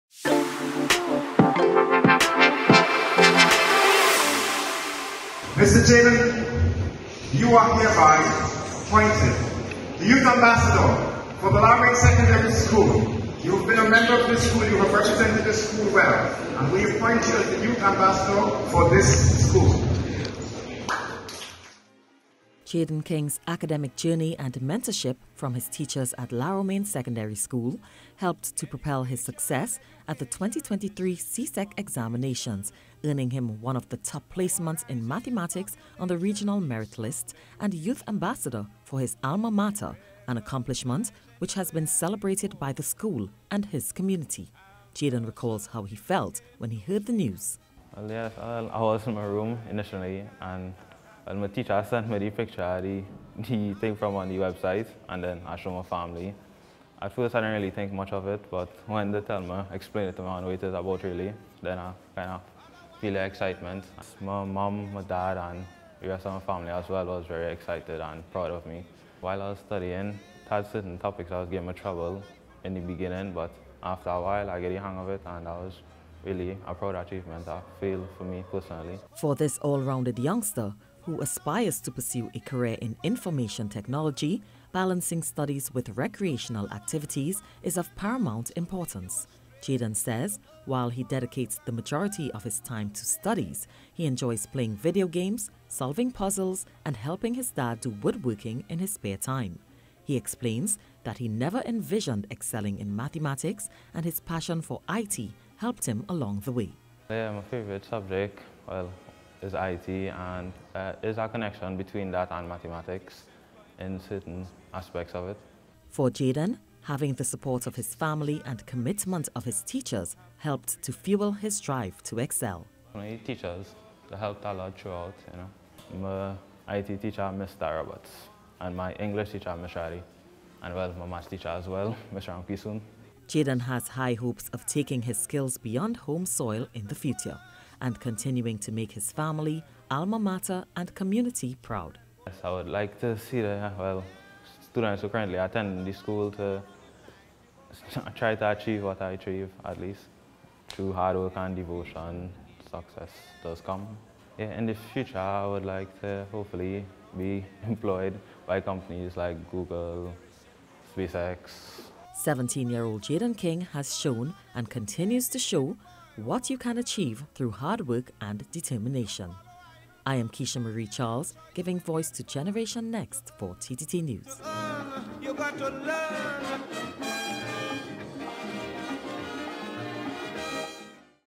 Generation Next: Jayden King
In tonight's Generation Next installment Keisha Marie Charles sheds light on the achievement of 17 year old Jayden King who exemplifies the meaning of working hard to gain success. This former student of La Romaine Secondary School who holds one of the top spots on the CSEC Mathematics regional merit list 2023, proves that with dedication and perseverance the sky is the limit!